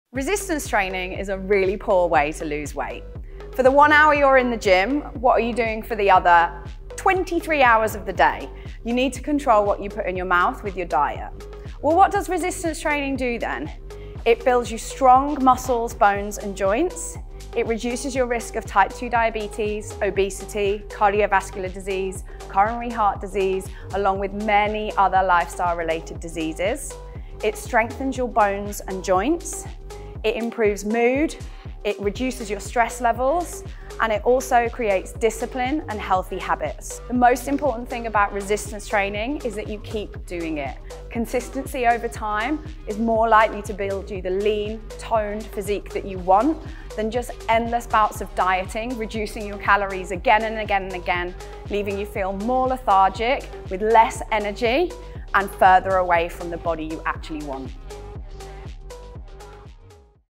Resistance training and weight loss - does it work?
What does resistance training do?

If you're solely looking for weight loss, it might not be the most effective method.

Instead, resistance training will help you gain muscle, strength, bone density and help you fight chronic disease such as coronary heart disease.